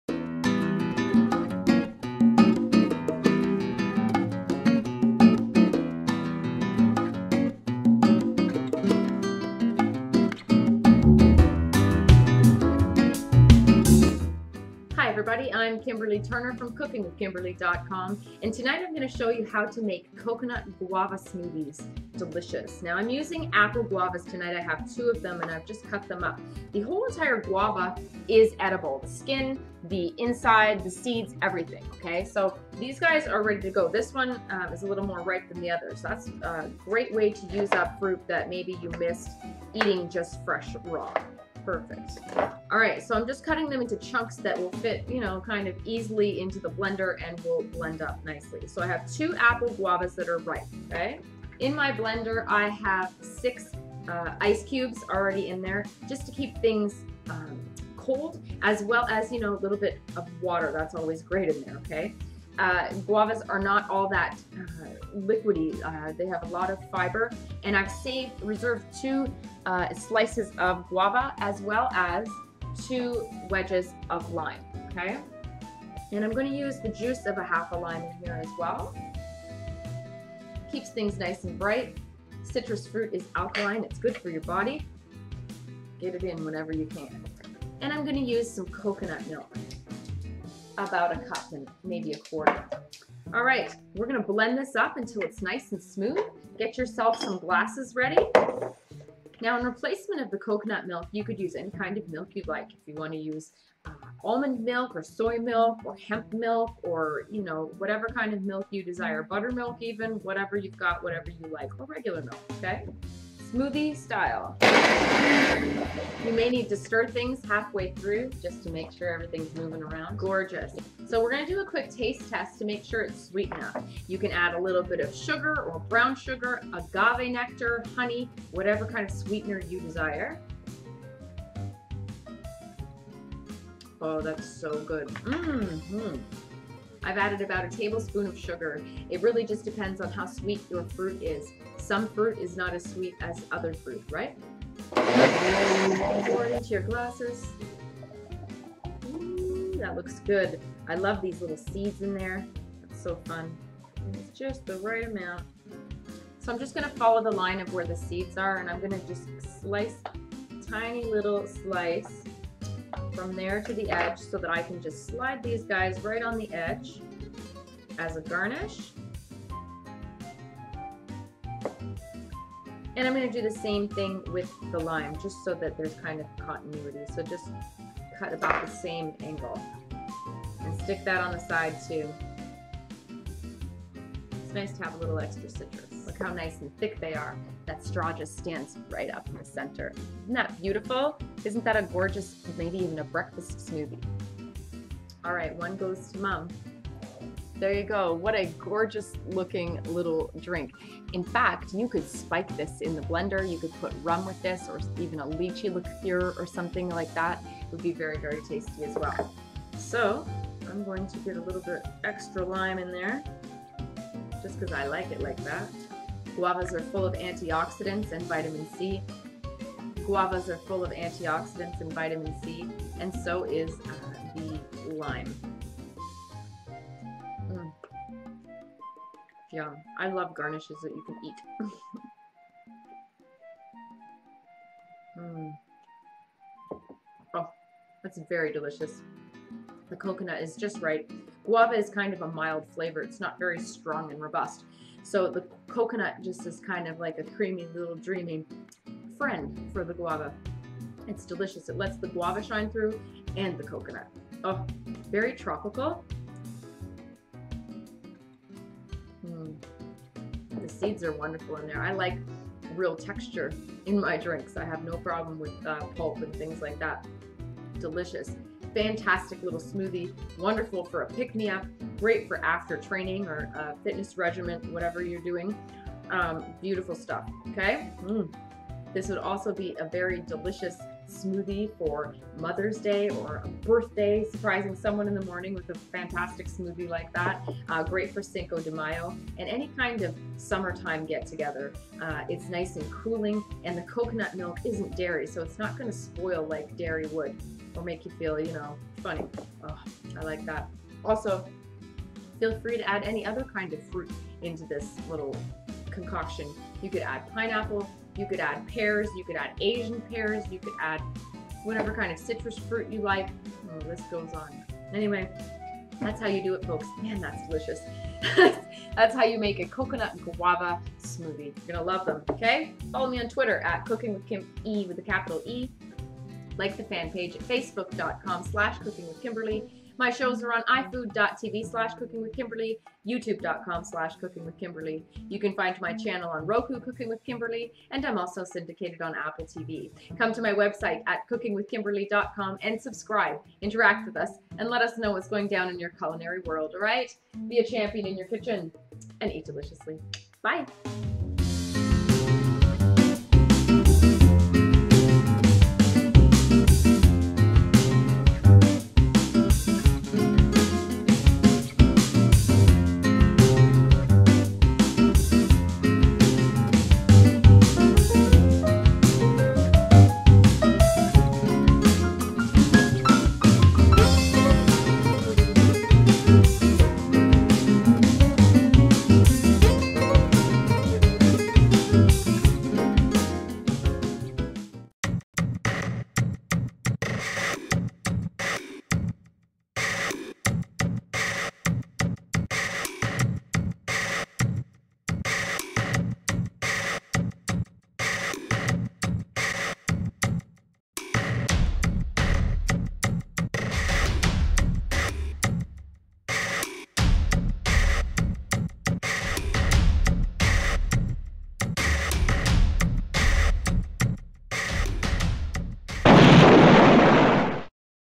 How to Make Coconut Guava Smoothies: Cooking with Kimberly
shares with you How to Make Coconut Guava Smoothies!

Kimberly's Fitness: "Don't Hate...Get in SHAPE!"